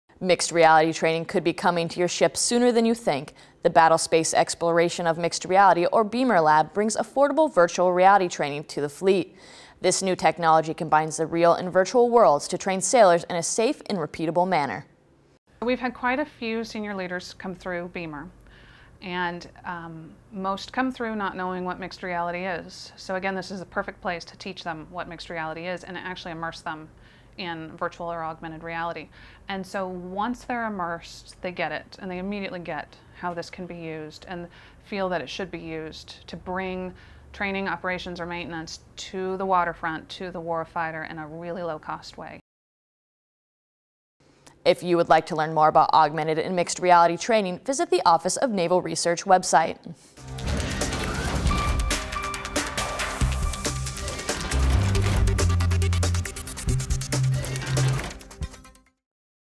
ONR Virtual Reality Part 2
Mixed reality might be coming to your ship sooner than you think.